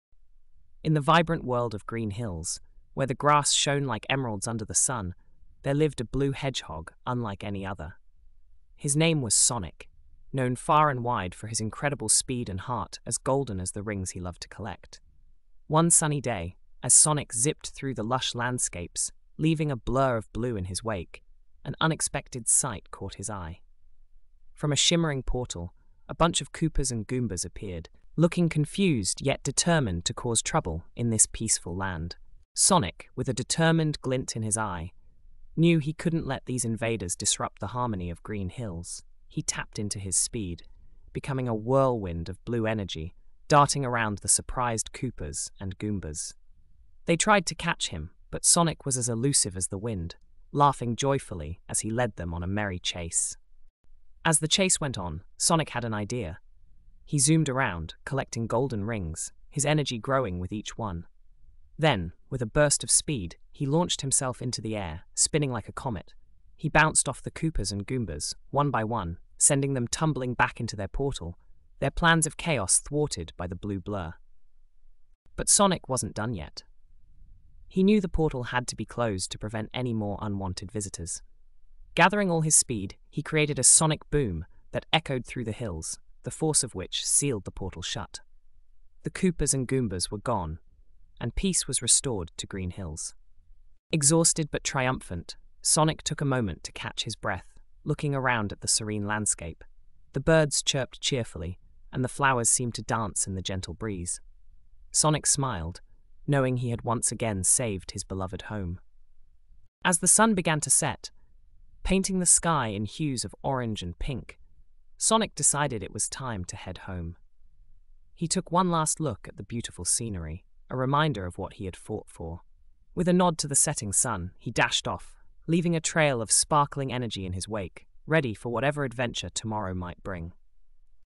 Sonic and the Invasion of the Portal Foes: A Green Hills Adventure
Prompt:
a blue hedgehog that looks like a human. his name is sonic. he was running around green hills and then a bunch of koopas and goombas showed up. tell me about the battle.

Storyteller:
A PS10 student, Brooklyn, NY

TaleTelr Version: alpha